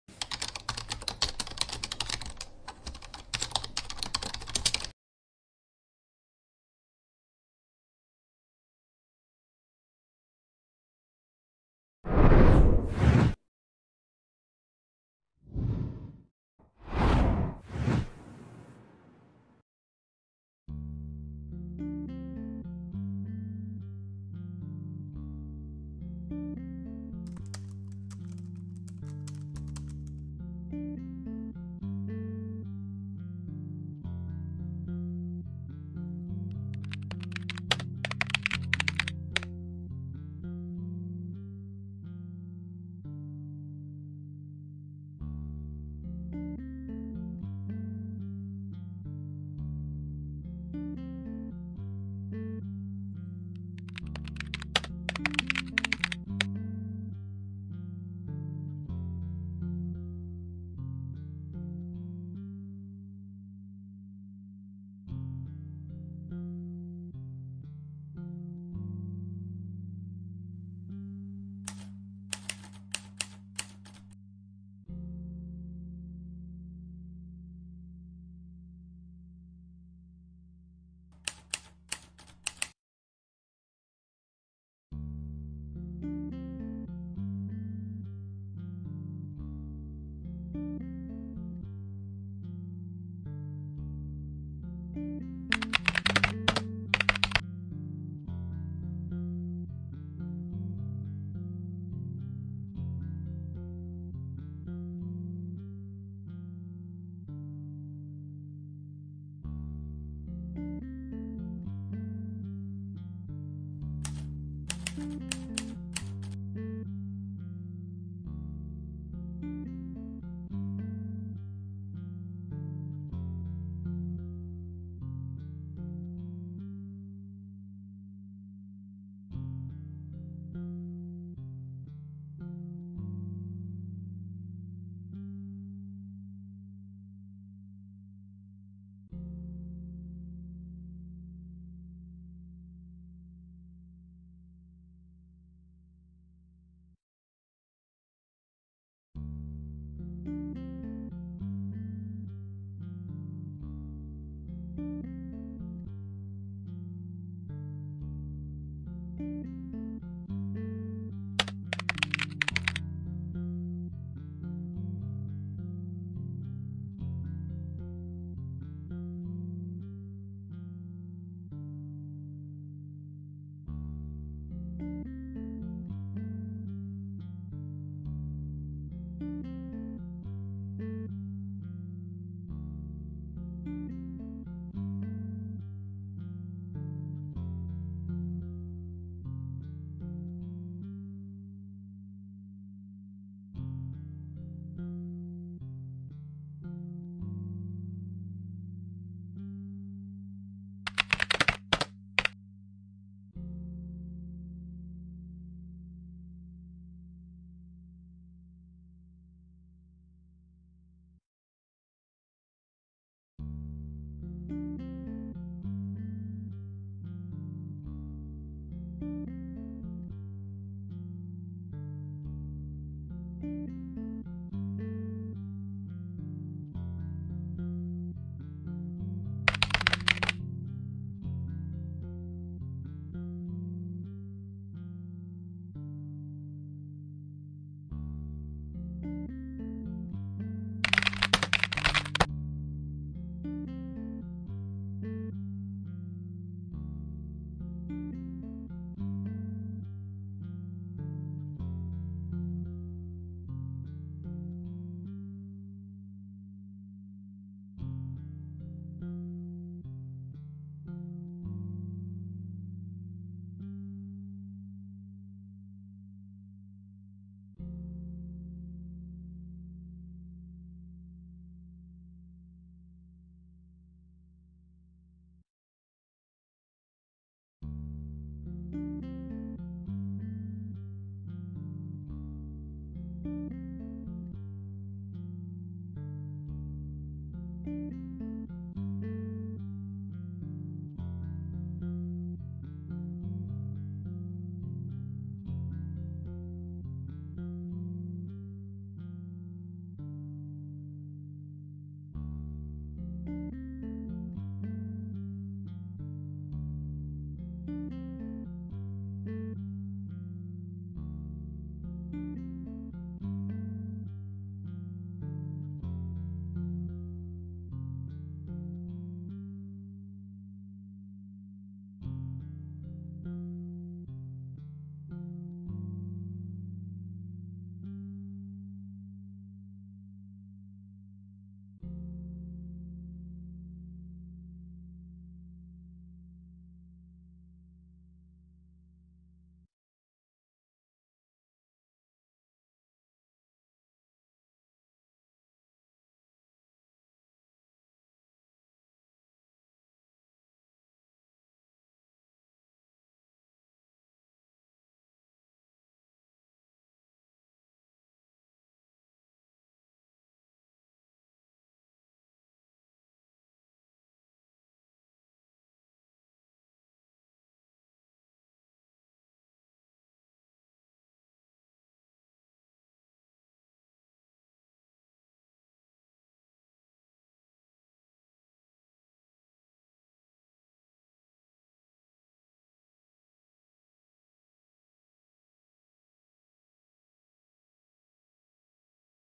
Cartoon to Realistic Monster Photo Editing with PS Touch Mobile
Join me on a journey from cartoons to real-life! In this video, I'll show you how I used the PS Touch mobile app to turn a fun cartoon monster into a lifelike creation. Watch step-by-step as I reveal the secrets behind making it look so real. You'll learn easy tricks to add depth and make your own creations come alive—all using just your phone! Let's explore the magic of PS Touch together and bring imagination to life!